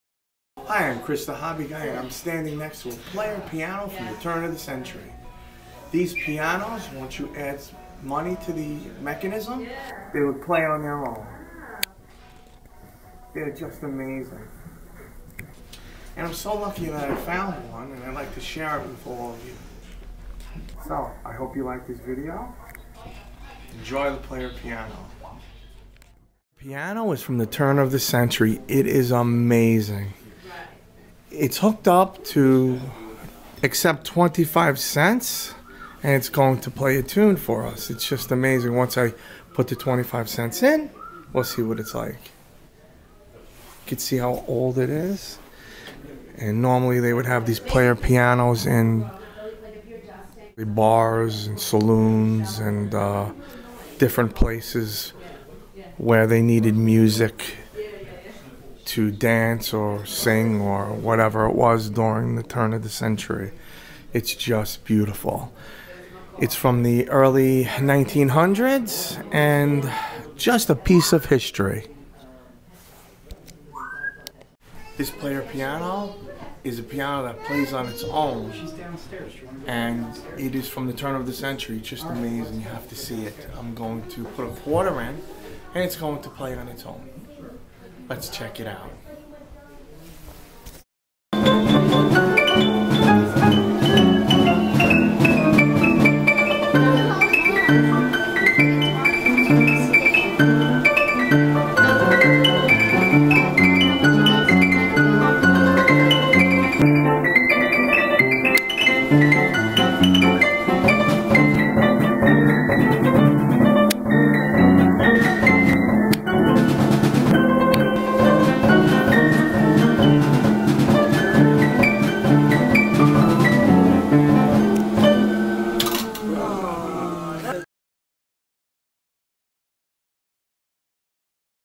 A Player Piano from the early 1900's
This is a Player Piano from the early 1900's. They are extremely rare and very valuable to collectors.